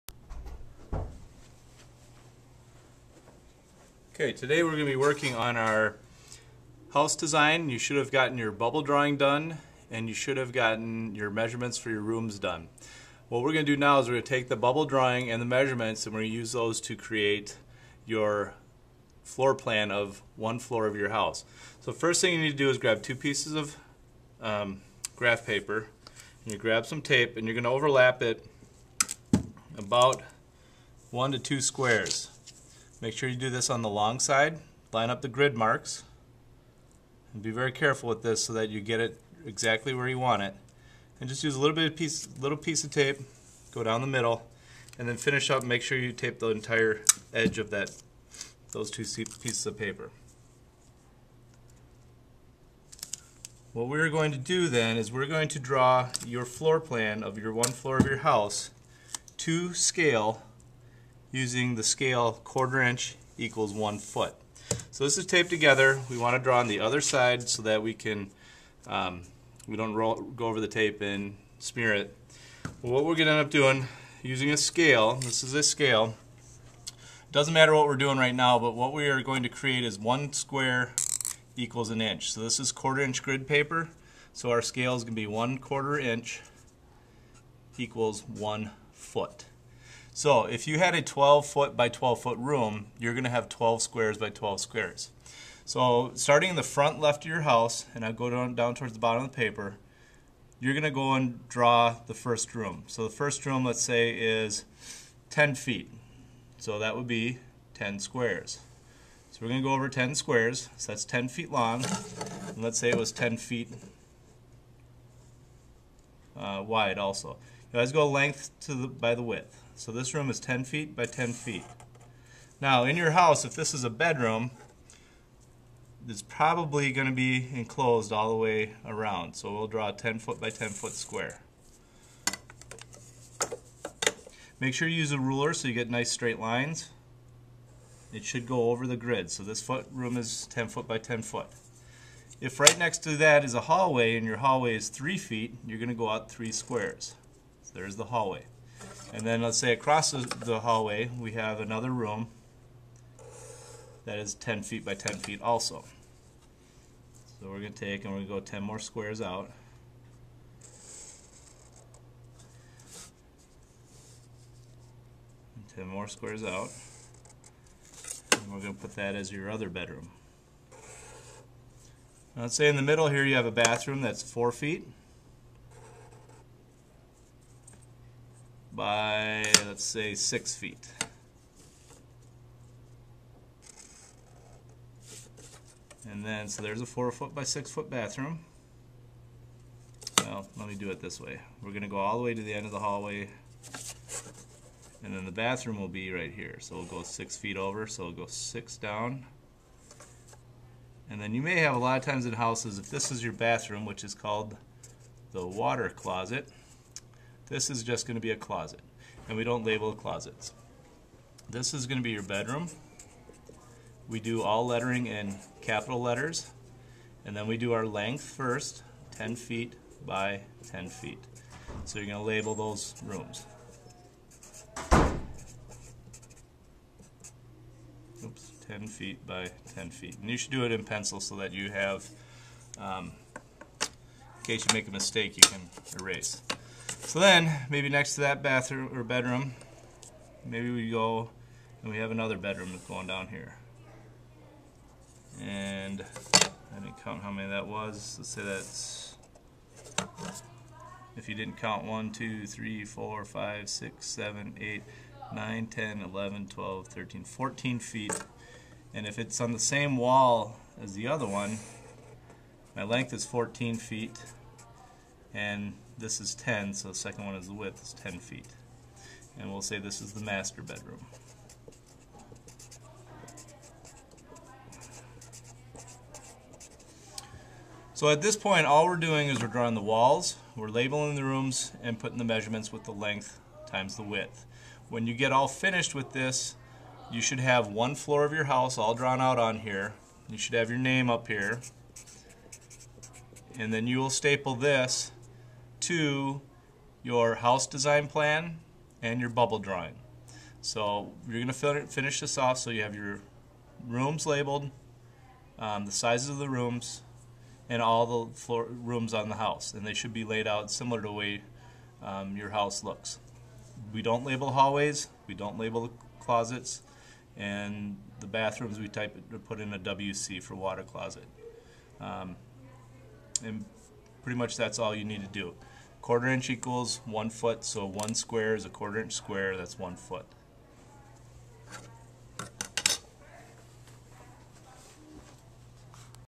scale drawing
Video shows how to draw a 1/4" = 1 foot scale drawing